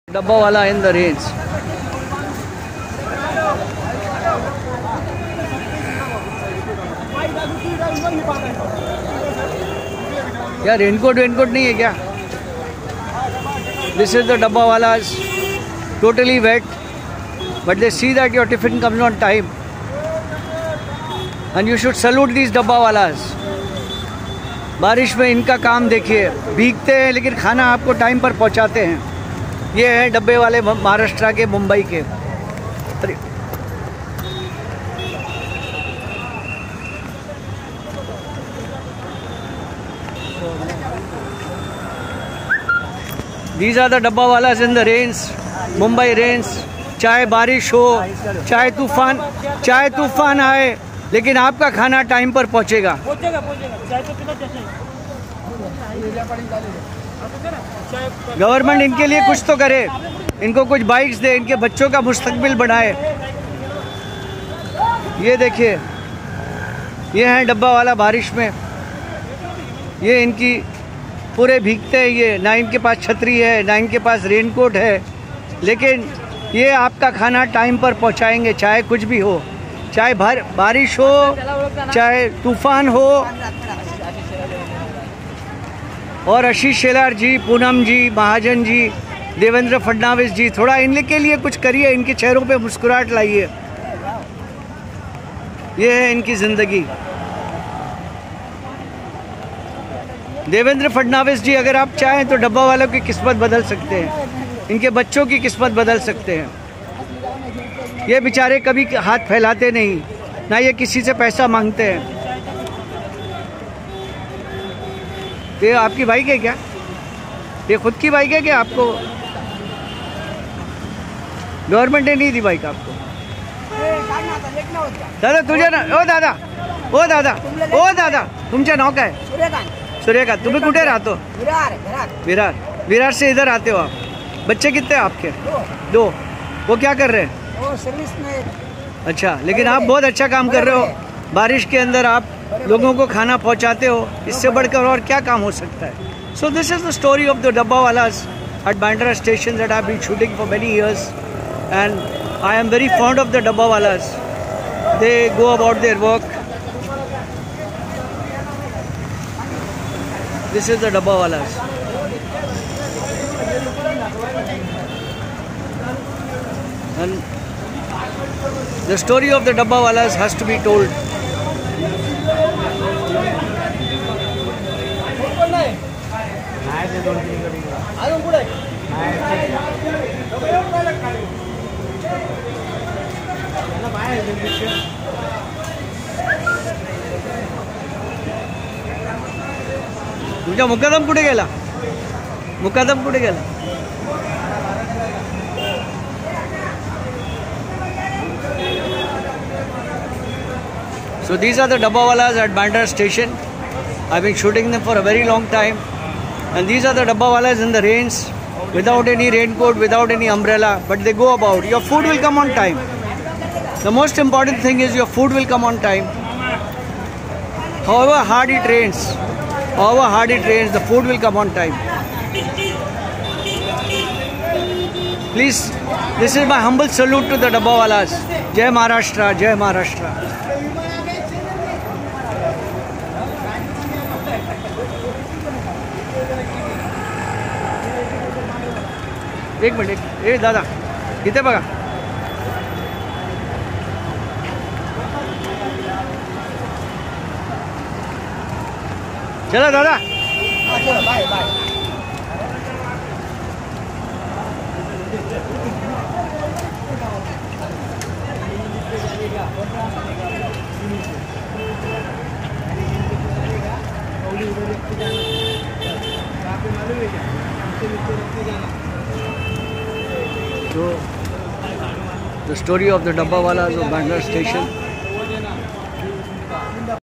Dabbawalas In the Rains at Bandra Station
I have been shooting these Bandra Station Dabbawalas since a very long time. But shooting them in the rains and watching their pain and plight is a tough task.
Mind you the Dabbawalas don't complain come rain or storm your tiffin with home food will reach your office on time.
The Dabbawalas don't bother about their own lunch.. That is secondary first  task for them is that your meal should reach you on time.

They are drenched to the bone their cycle breaks down.. Do offer them your meals this would be a beautiful gesture.
Take care of them they are our humble dedicated brothers.
Jai Maharashtra
Dabbawalas pride of Mumbai.

This channel subscribes to Original Content.